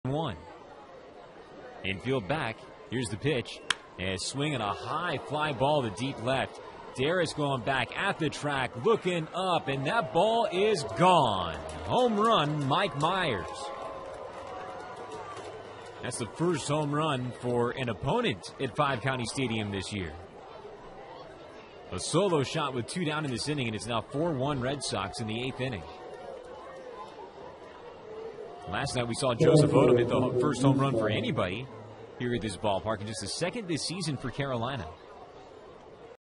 Meyers rips solo shot for Salem
4/20/16: Salem's Mike Meyers slugs his first homer of the year as the Red Sox top Carolina, 5-1